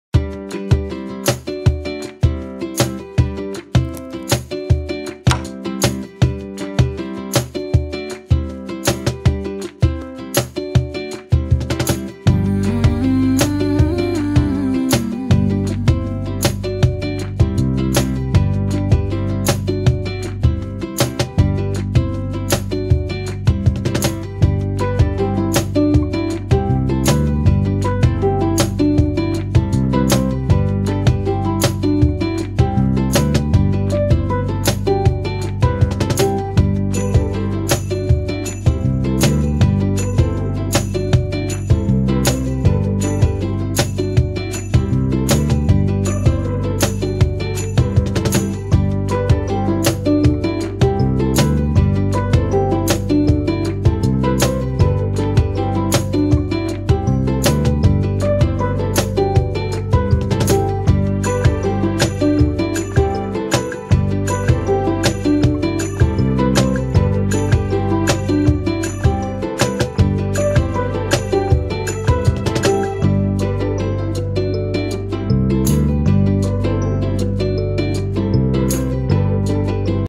Magic and summer vibes with Hermione at the beach! Affinity Photo speed up process
So, This summer I took just a couple of days of vacation, so I sent Hermione to enjoy the sea for me (thanks to affinity photo), haha

🎵  Music 🎵

With love
Ekira  ༼ つ ◕_◕ ༽つ